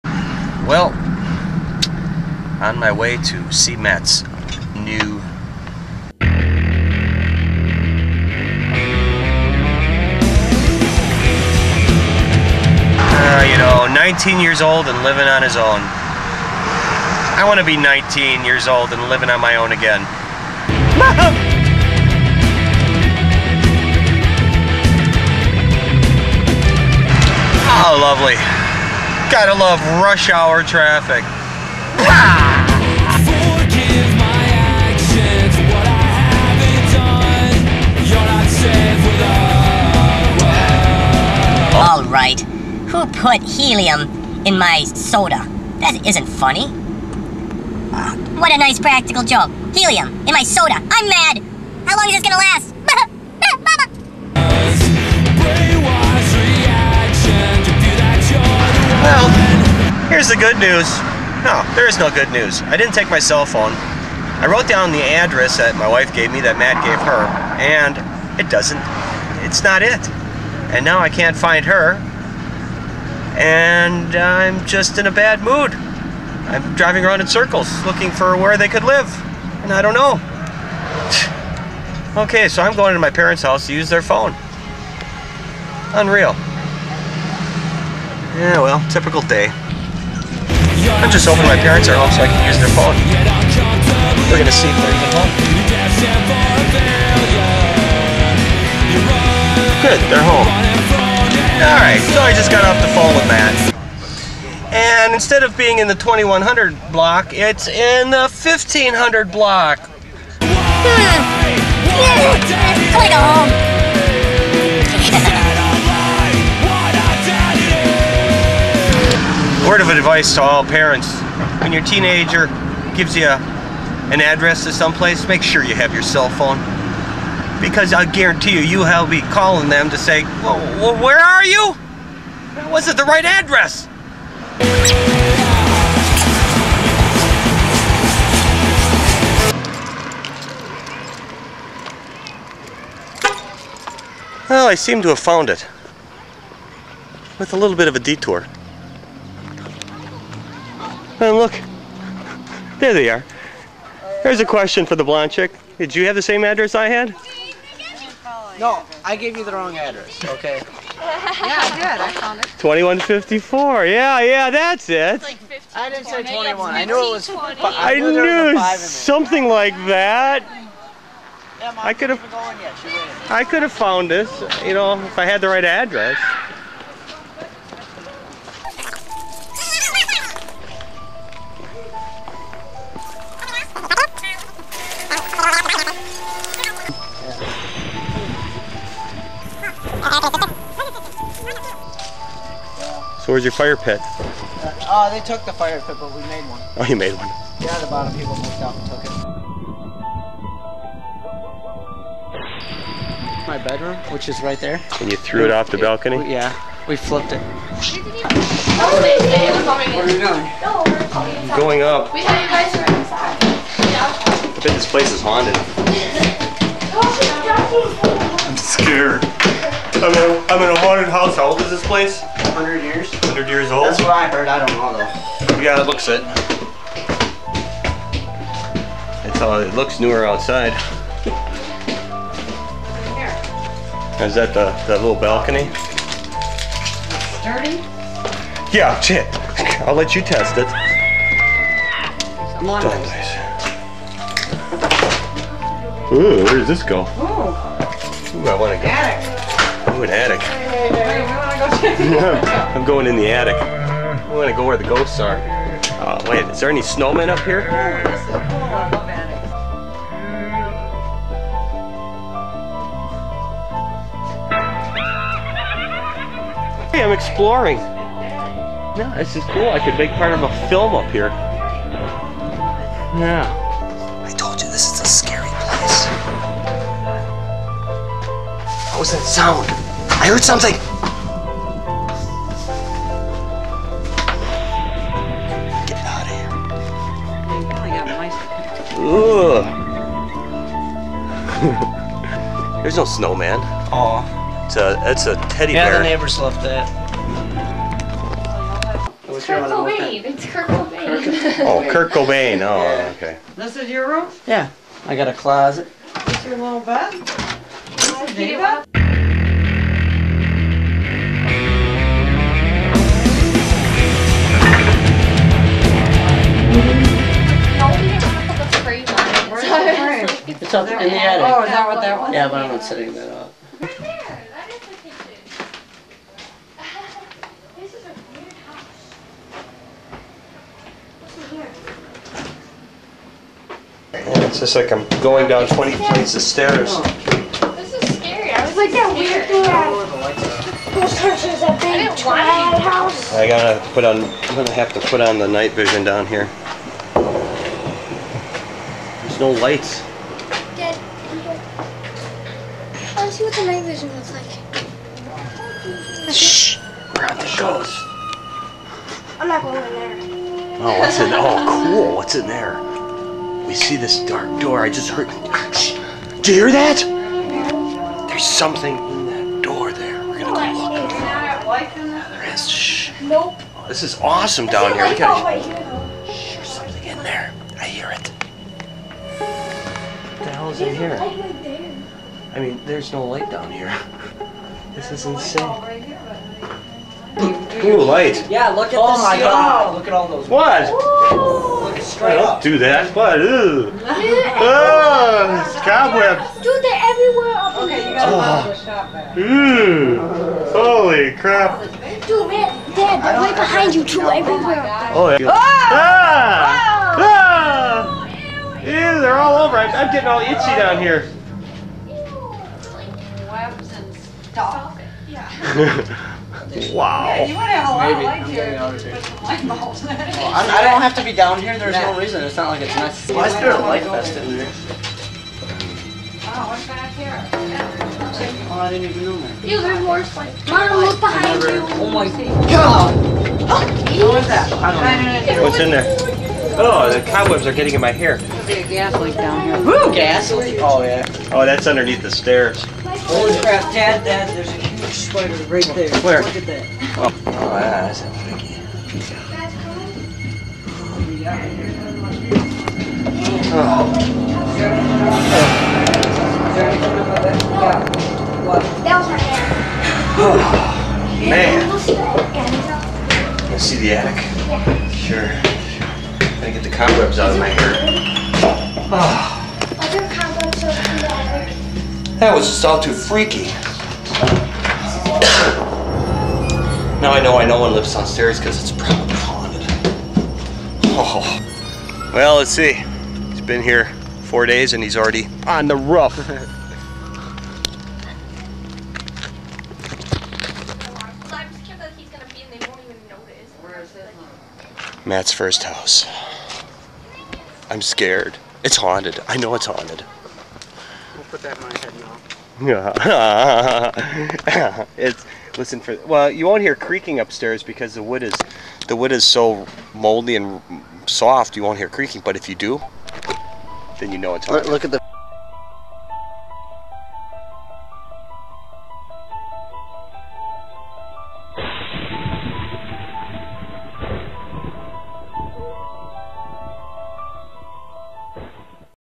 Scary - Matt's scary New Home
Well, he up and moved out. And this place looks perfect for a spooky night.